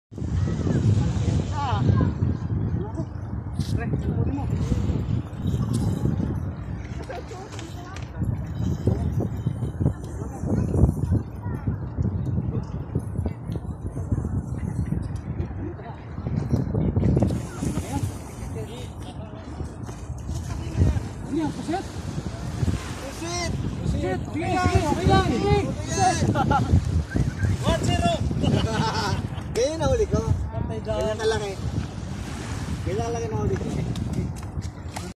3 Kilos TALAKITOK Pain MATAMBAKA Corniche Red Sea Jeddah KSA / Continuation (Part 2)
Ito na po nanaman yung nahuling Talakitok na tatlong kilos.